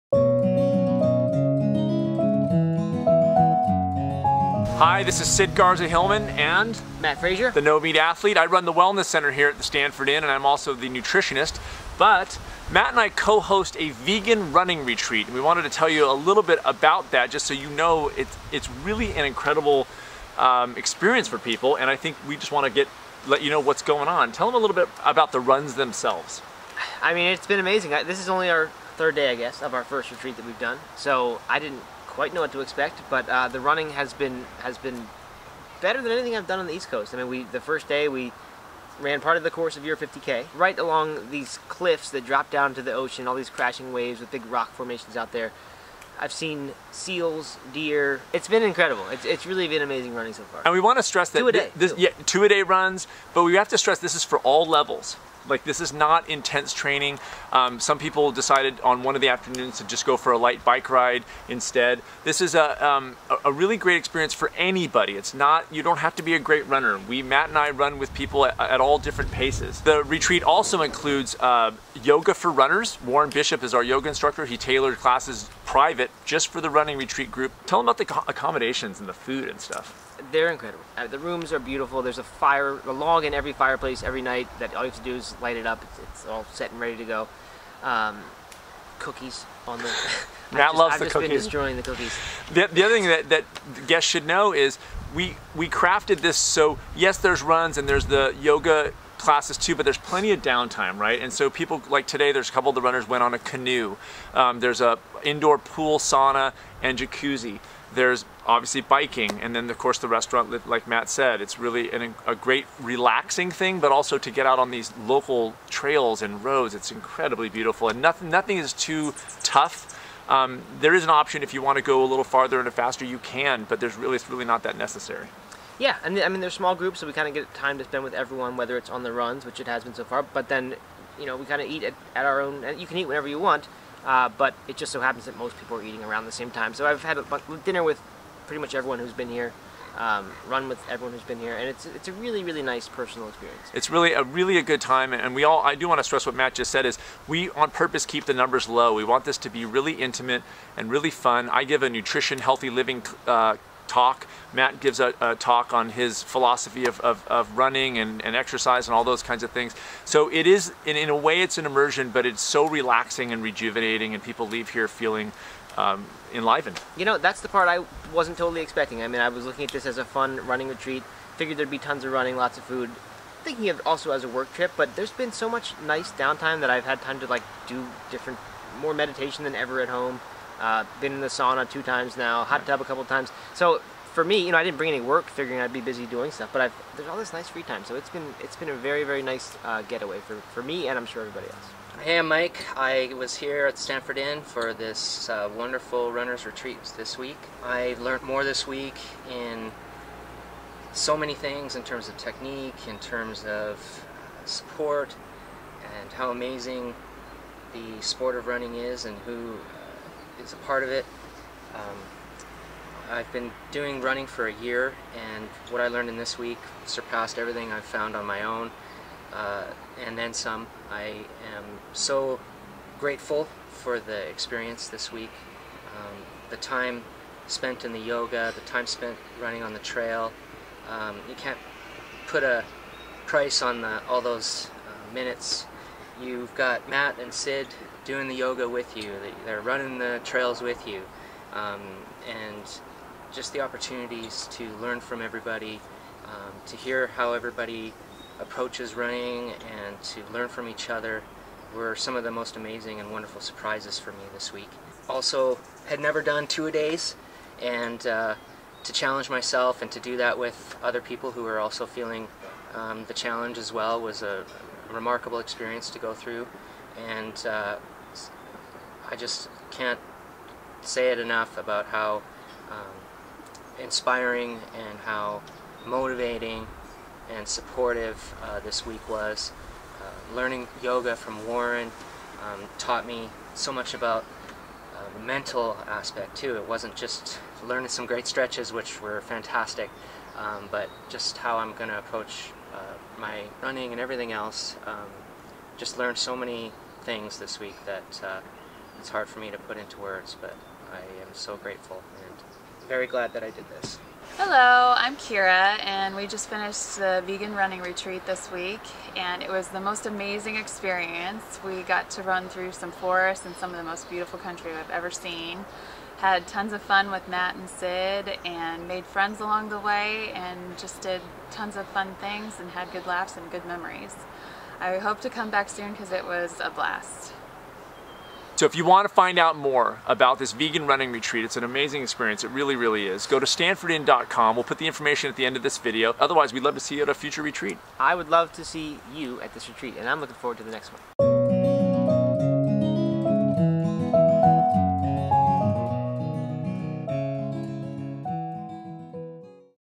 Stanford Inn's Vegan Running Retreat
Learn about Stanford Inn's incredible running retreats! All running levels welcome and whether you're vegan or not this is an incredible experience!!! Just great food, fun, and beautiful running...